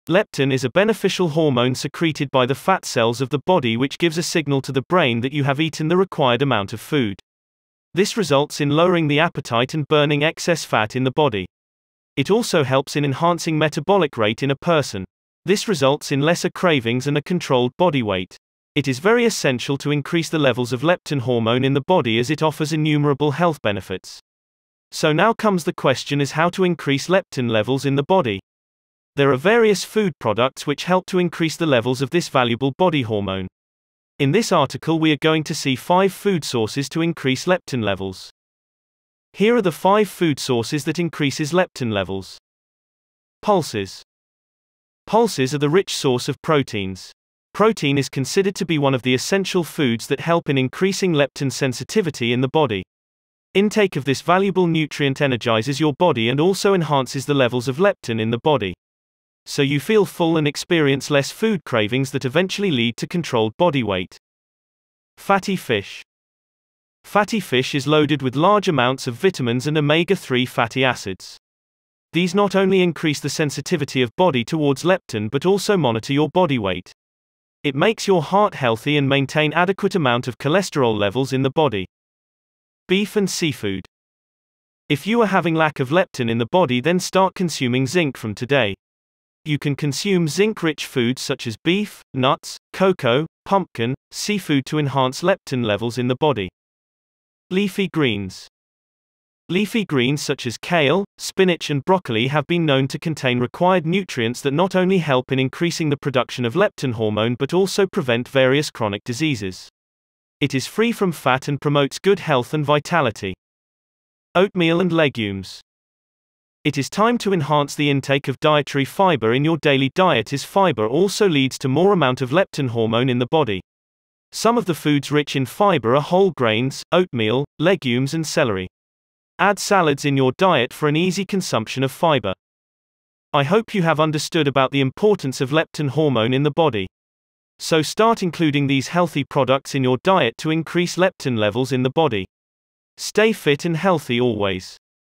5 Top Leptin Inducing Food Sources
Leptin is a beneficial hormone secreted by the fat cells of the body which gives a signal to the brain that you have eaten the required amount of food. This results in lowering the appetite and burning excess fat in the body. It also helps in enhancing metabolic rate in a person. This results in lesser cravings and a controlled body weight. It is very essential to increase the levels of leptin hormone in the body as it offers innumerable health benefits. So now comes the question as how to increase leptin levels in the body? There are various food products which help to increase the levels of this valuable body hormone.